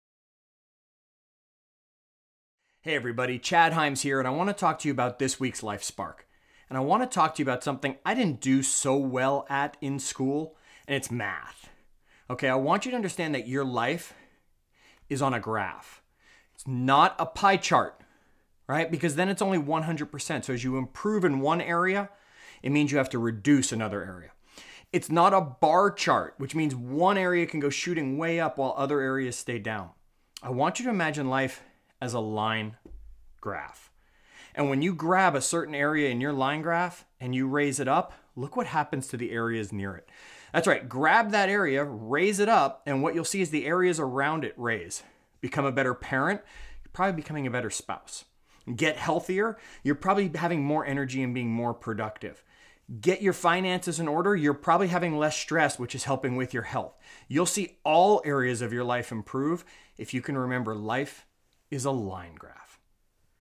Life Spark Line Graph
Life is not a pie chart or a bar graph, it is a line graph. Improve one area and watch the others climb!